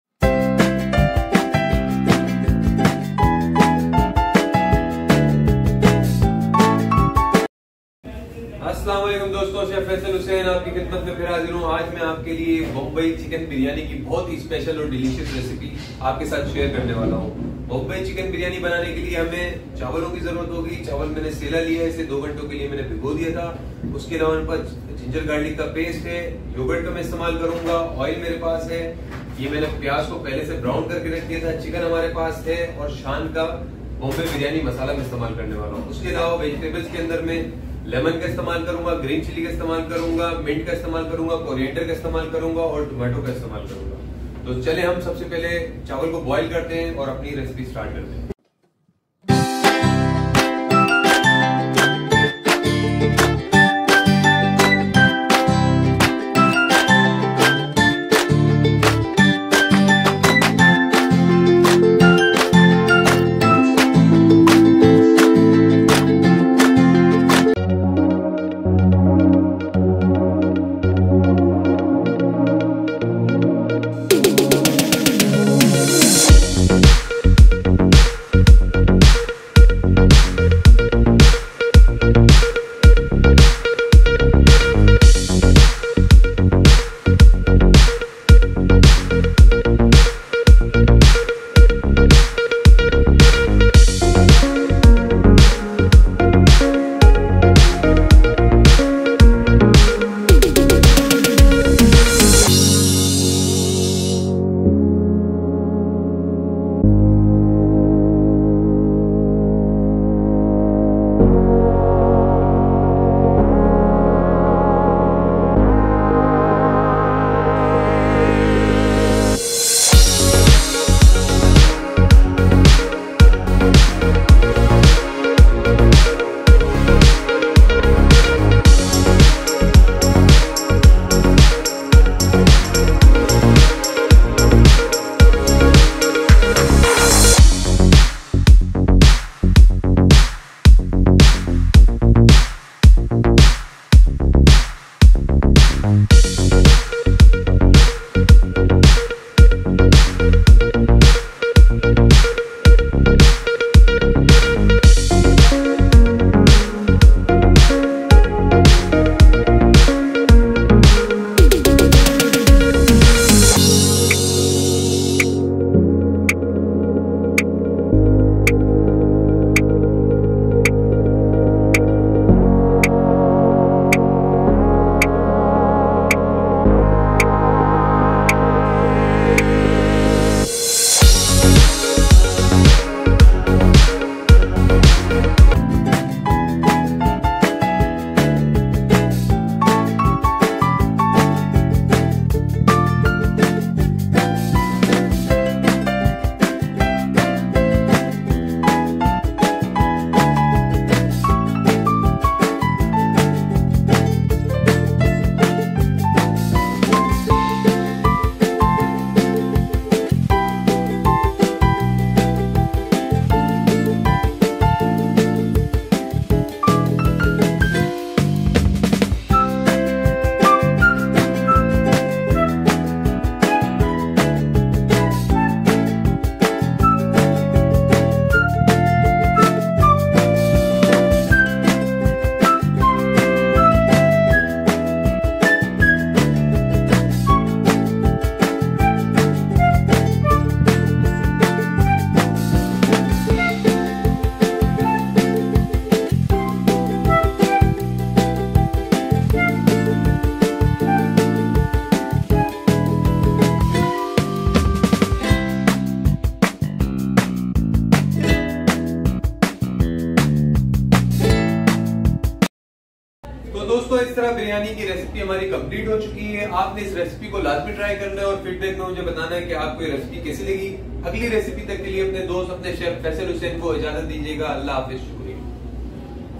Orignal famous Bombay Biryani Recipe | Special Chicken Birynai | Chef Faisal Hussain
Bombay Chicken Biryani

INGREDIENTS:
Chicken                         :                 1 kg
Yogurt                           :                  1 cup whipped
Biryani masala             :                 1 pkt
Oil                                  :                 1/2 cup
Rice                               :                 750 g
Onion                            :                  2 large
Tomato                         :                  270 g
Ginger Garlic                :                 3 tbsp paste

For Topping :
Tomato                         :                  1 each
Fried Onion                   :                 1 cup
Mint                               :                 1/4 cup
Corriender                    :                 1/2 cup
Green chilli                   :                  4 to 6 pcs
Lemon                           :                  1 each